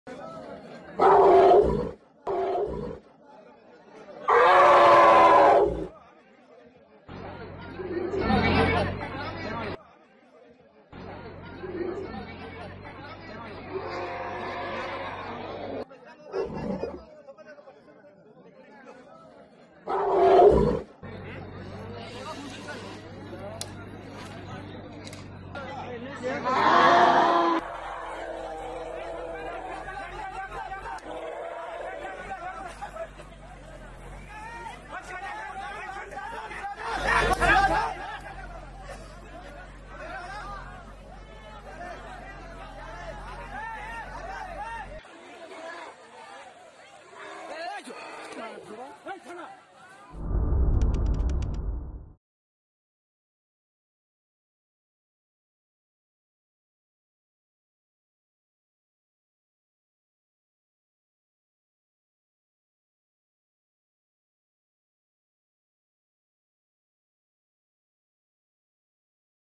Rescatan en la India a una cría de elefante que había caído al fondo de un pozo
Tan pequeño y ya ha tocado fondo. Pero esta cría de elefante es más que eso. En la India, el dios Ganesh, con cabeza de este animal, es uno de los más venerados de la religión hinduista. Adoración que extienden a cada todos los ejemplares de este mamífero proboscídeo. Así que en esta aldea del sureste del país no tardan en poner en marcha la misión rescate. Después de horas de angustia para el pequeño elefante, que durante la noche se había despistado de la manada y caído al pozo, logran poner fin a su odisea.